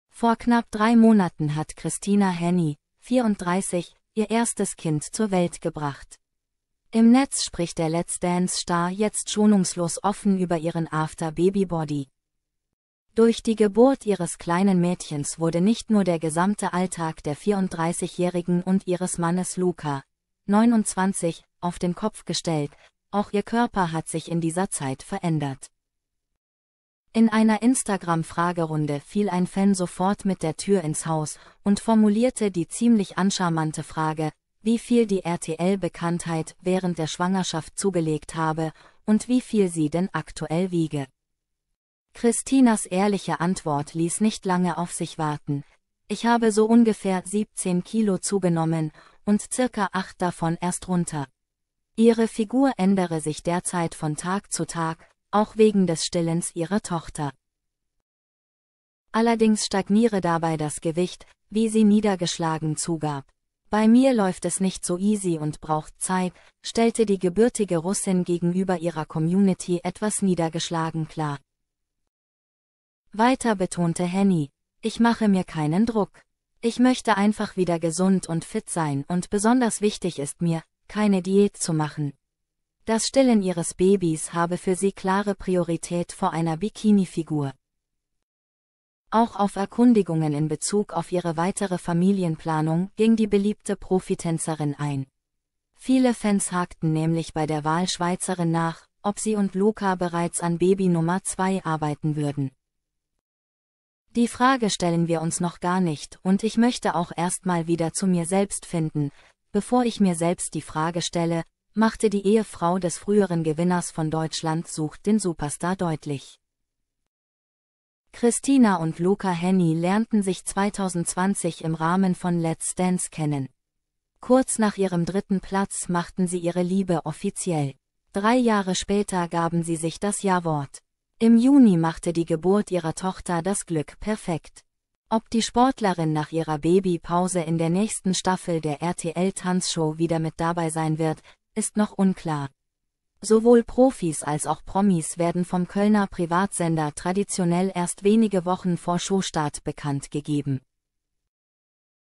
Offene Worte von Christina Hänni: "Let's Dance"-Star hat mit After-Baby-Body zu kämpfen!
Vor knapp drei Monaten hat Christina Hänni (34) ihr erstes Kind zur Welt gebracht.